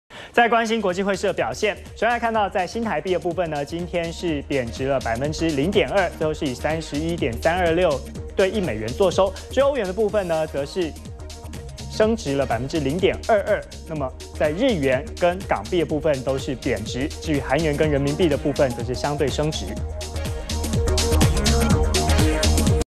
【2014.12.12】國際匯市 -udn tv
關心台灣時間12月12日的全球匯市動態。完整內容 , FB , Twitter , G+